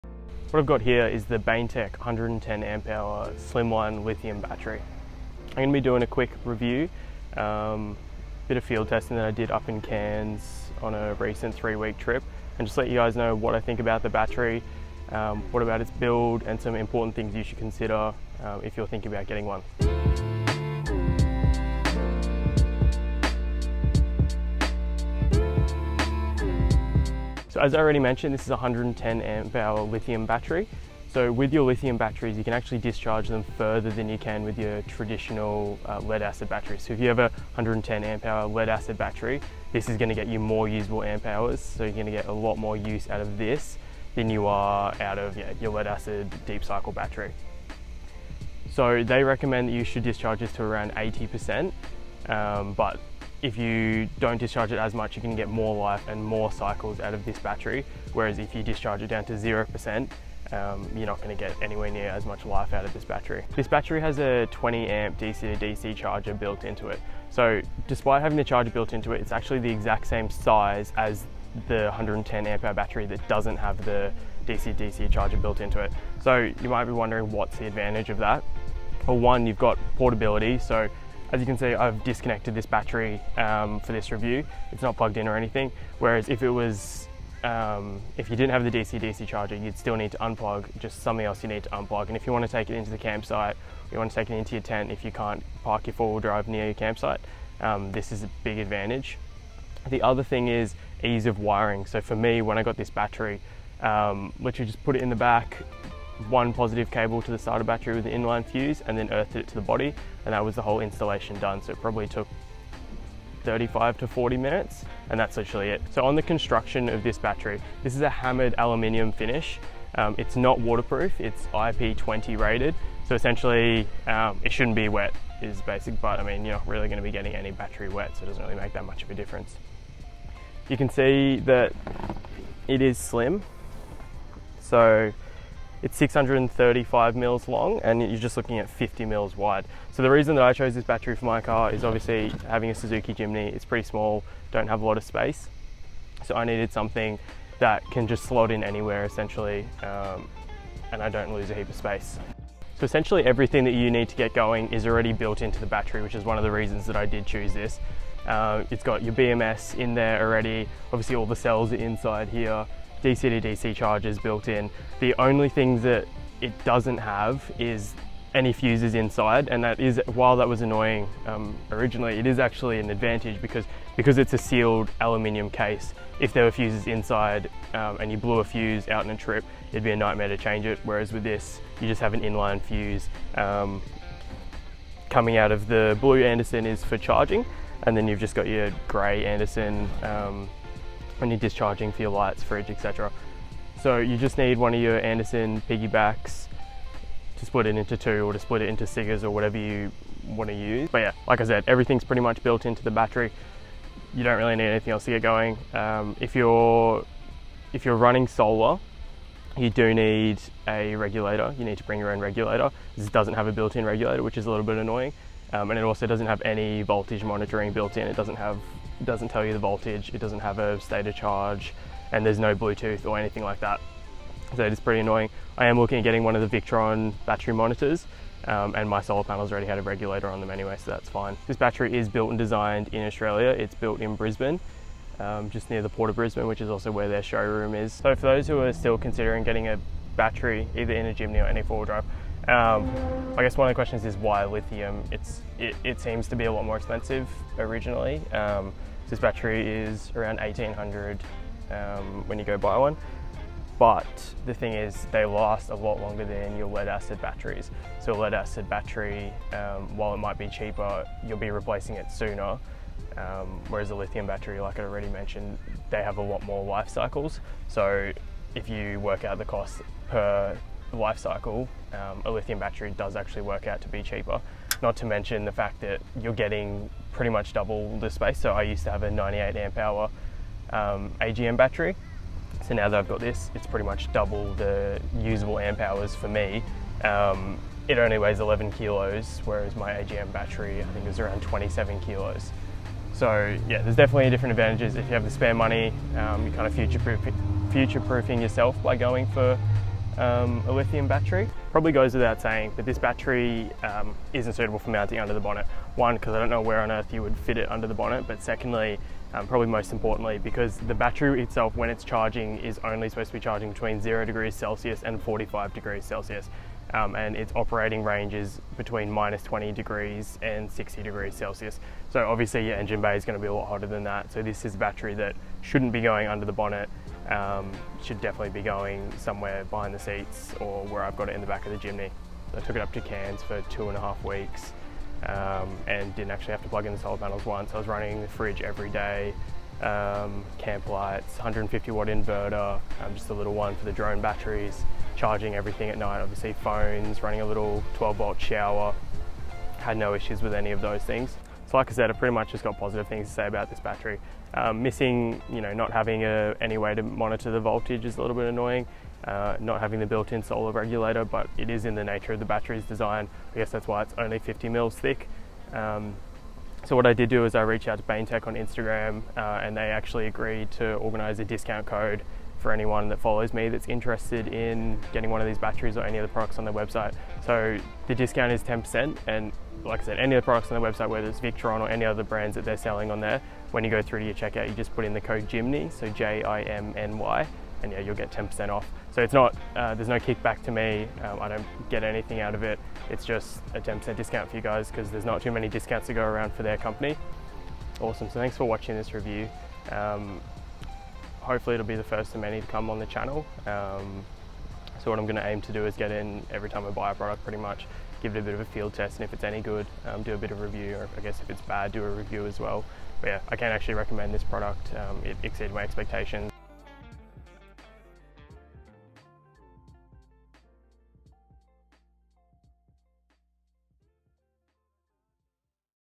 Baintech 110ah Slimline Lithium Battery REVIEW
Here is my in-depth review of the Baintech 110ah Slimline Lithium Battery with built in DCDC charger.

Please note: Since filming this video I have been made aware the wiring for the Victron BMV-712 Smart battery monitor differs from the suggested wiring instructions given by Victron, and only offers approximately 90% accuracy.